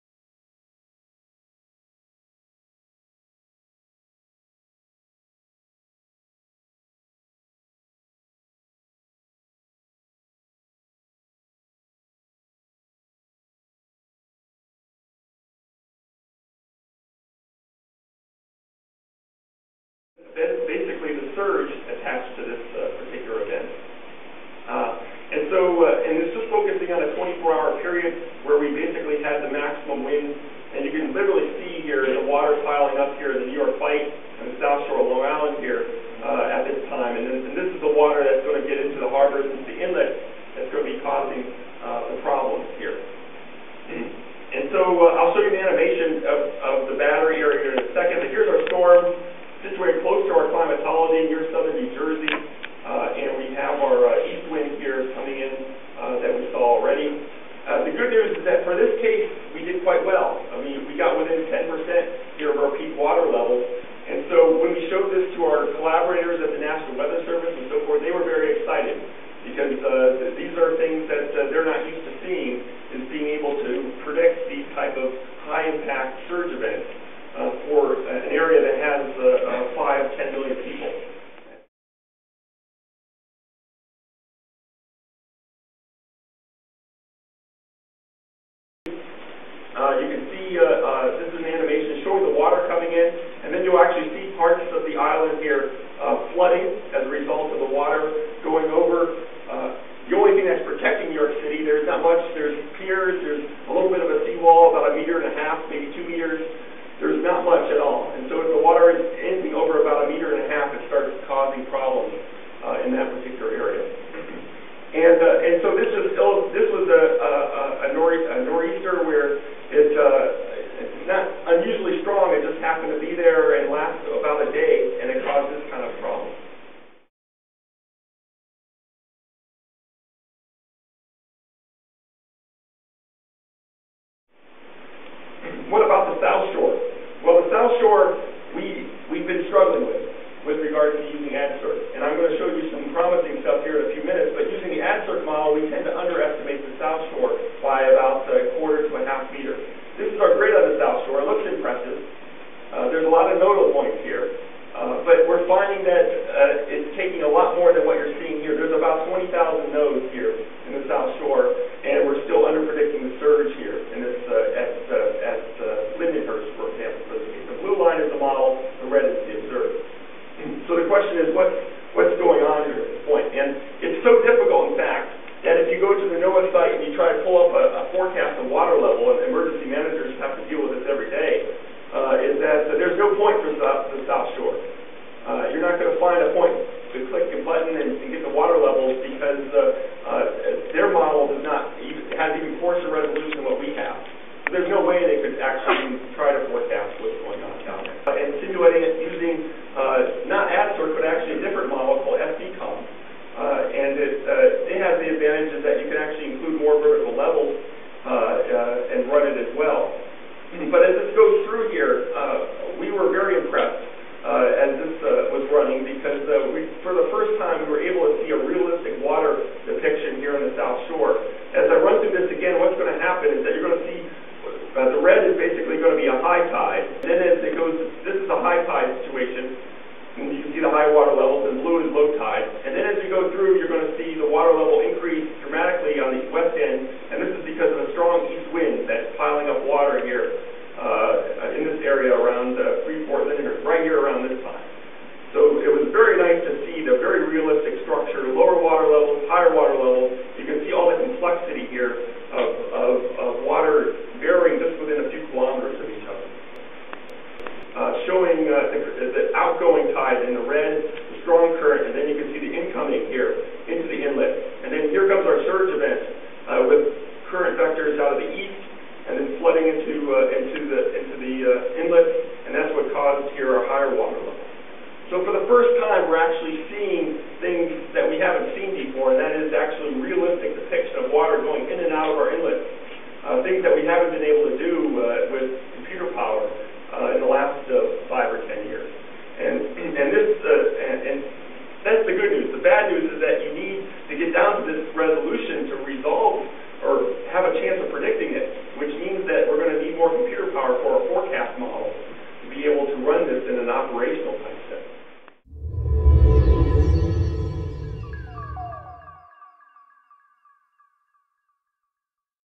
Storm Surge Modeling (December 2008)
December 2008 presentation on "Storm Surge Modeling in NYC" at Stony Brook Southampton by Dr. Brian Colle School of Marine and Atmospheric Sciences Stony Brook University